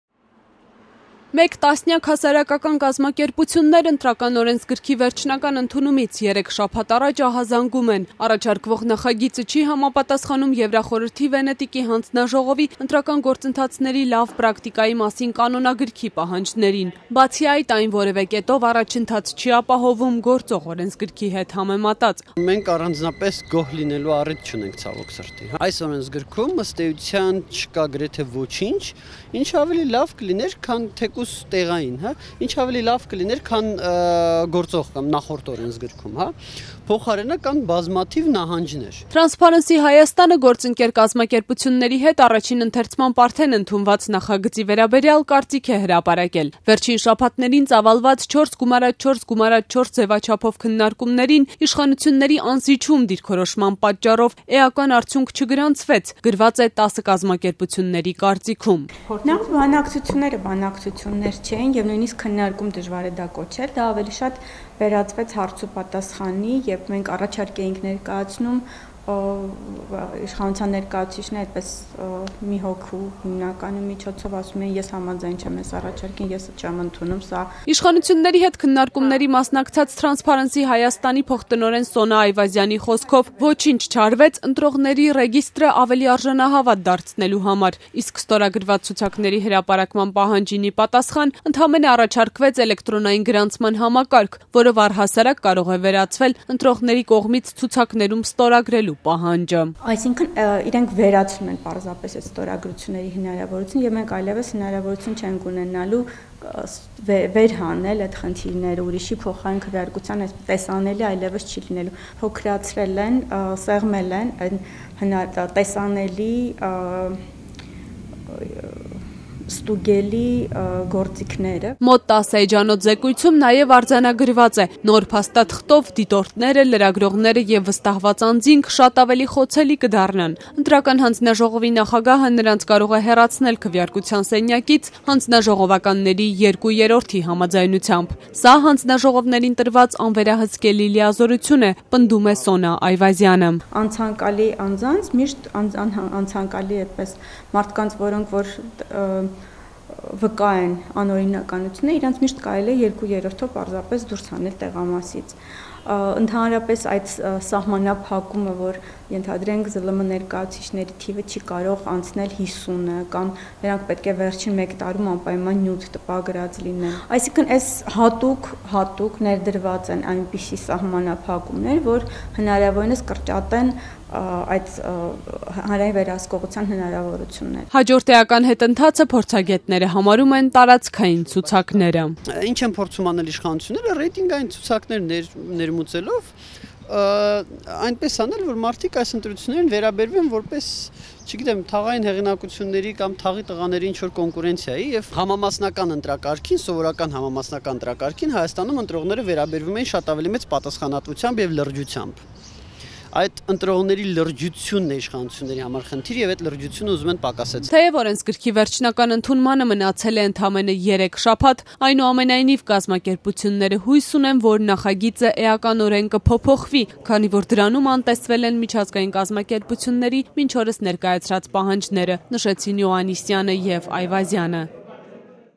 ՀԿ-ների կարծիքով, նոր ԸՕ-ի մշակման գործընթացը չի համապատասխանել միջազգային պահանջներին
Հասարակական կազմակերպություններն, այդուհանդերձ, դեռ հույս ունեն, որ մինչև վերջնական ընդունումը, նոր ԸՕ նախագիծը «էականորեն կփոփոխվի»: